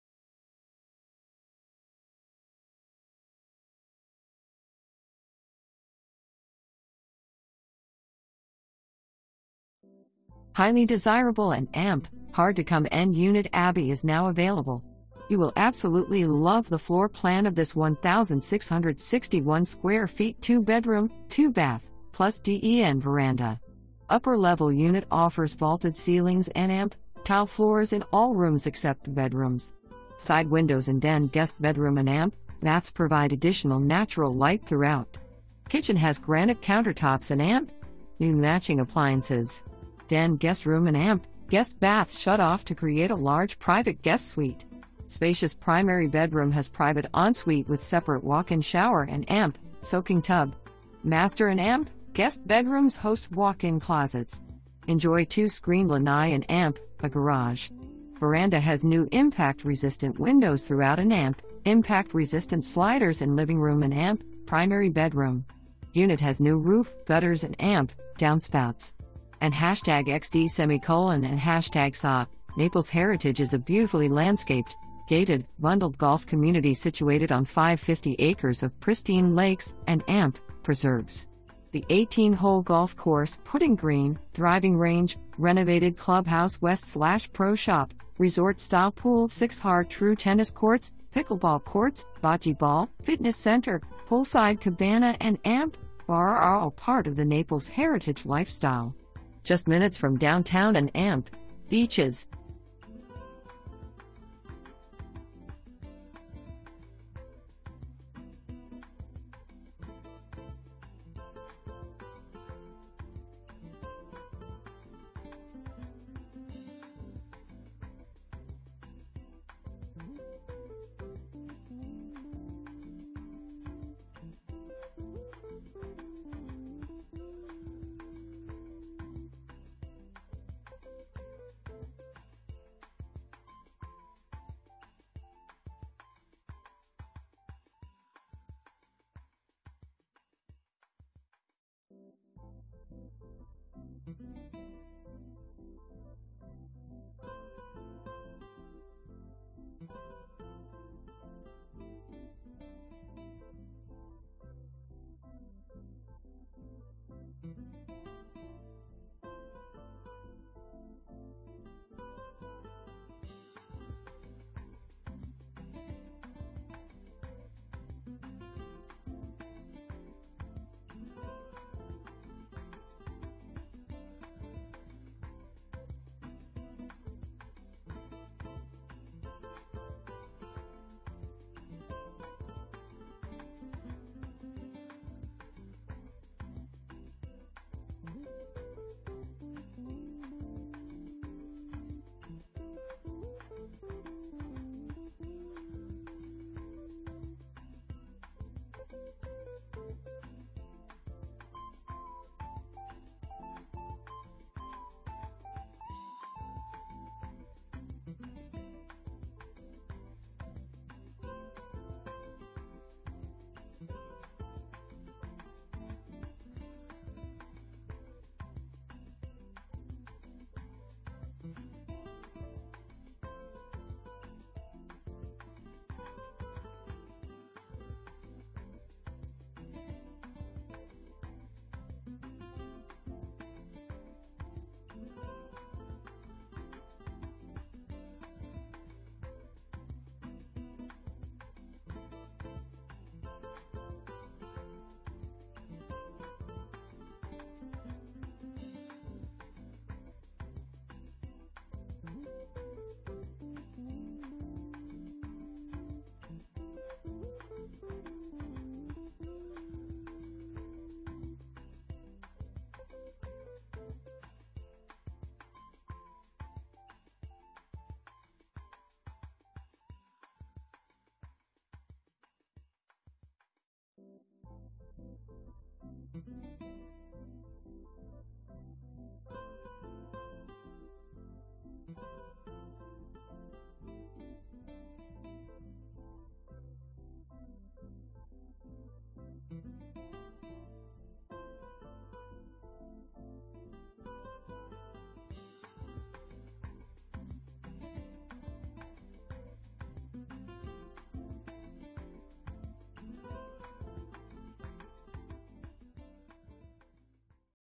Berkshire Hathaway HomeServices Florida Realty - 8370 Heritage Links CT 2026
8370 Heritage Links CT 2026

HIGHLY DESIRABLE &amp; HARD TO COME END UNIT ABBEY IS NOW AVAILABLE! You will absolutely love the floor plan of this 1661 sq ft 2 BR, 2 BATH, + DEN Veranda. Upper level unit offers vaulted ceilings &amp; tile floors in all rooms except bedrooms. Side windows in den, guest bedroom &amp; baths provide additional natural light throughout. Kitchen has granite countertops &amp; NEW matching appliances. Den, guest room &amp; guest bath shut off to create a large private guest suite. Spacious primary BR has private ensuite with separate walk-in shower &amp; soaking tub. Master &amp; guest BR's host walk-in closets. Enjoy 2 screened lanais &amp; a garage. Veranda has new impact resistant windows throughout &amp; impact resistant sliders in living room &amp; primary bedroom. Unit has new roof, gutters &amp; downspouts. &xD;&xA;Naples Heritage is a beautifully landscaped, gated, bundled golf community situated on 550 acres of pristine lakes &amp; preserves. The 18 hole golf course, putting green, driving range, renovated clubhouse w/ pro shop, resort style pool, 6 Har Tru tennis courts, pickleball courts, bocce ball, fitness center, poolside cabana &amp; bar are all part of the Naples Heritage lifestyle. Just minutes from downtown &amp; beaches.

Kimberly Vaughan
Berkshire Hathaway HomeServices Florida Realty